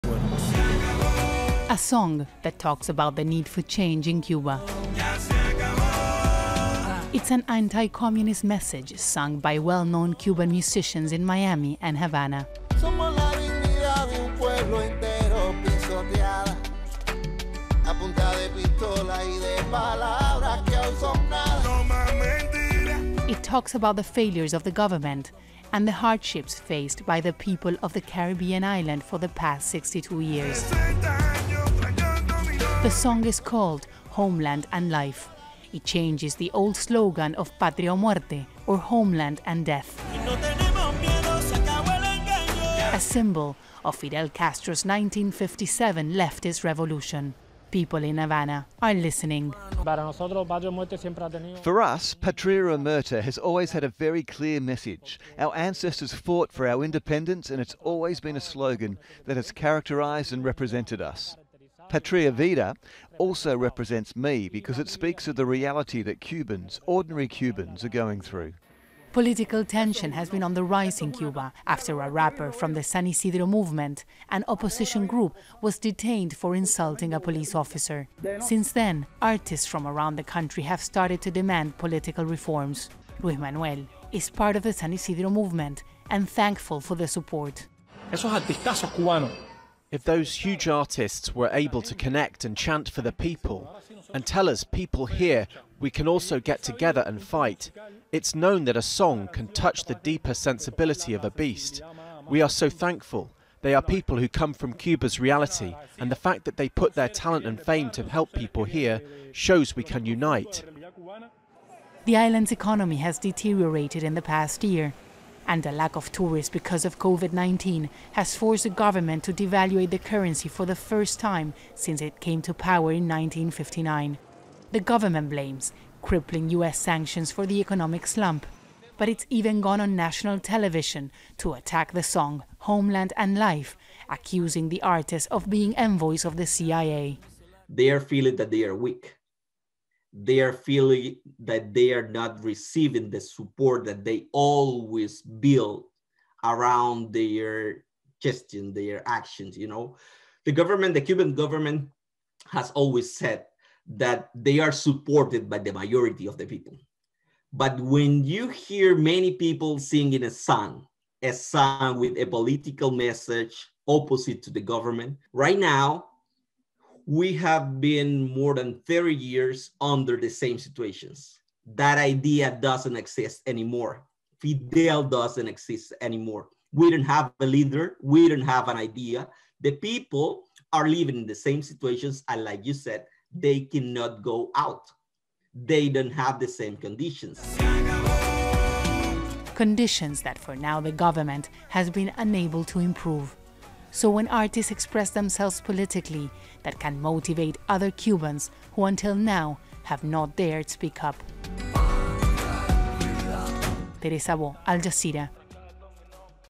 Cuba music: Anti-communist song goes viral
A song about hardship under communism has gone viral in Cuba, something that would have been unheard of when Fidel Castro was in power.
Some of the country's biggest music stars collaborated on the project, using lyrics that are direct and provocative.
Analysts say it is proof times are changing.
Al Jazeera's Teresa Bo reports.